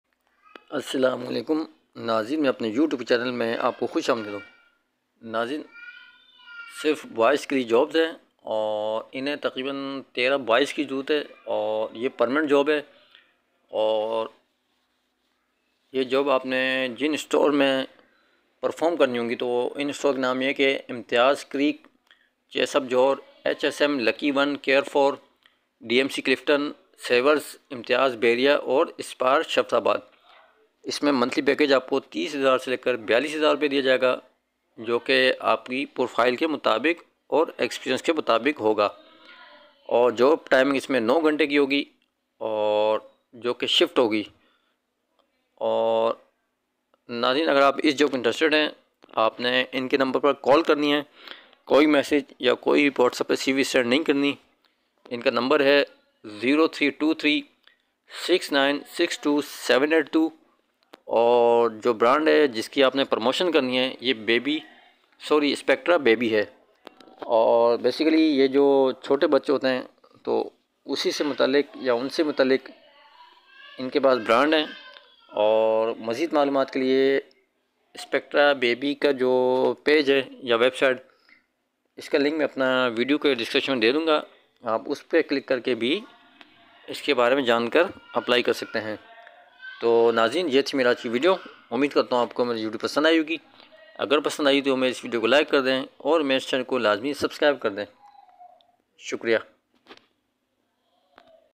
Need 13 Boys Permenant job For Imtiaz Creek, Chaseup Johar Lucky One Carrefour etc,Salary 42,000
*Only For Boys*

We Need 13 Boys.
Permanent Job

 *Store List* :
1) Imtiaz creek
2) chaseup johar
3) HSM
4) lucky one carrefour
5) DMC Clifton
6) saverz
7) Imtiaz bahira
8) Spar Sharfabad

*Monthly Package* 30,000 to 42000 Depends on Profile and Experience

*Job Timing:*  9 Hours  Shift*

Note: Only Call

Brand. Spectra BABY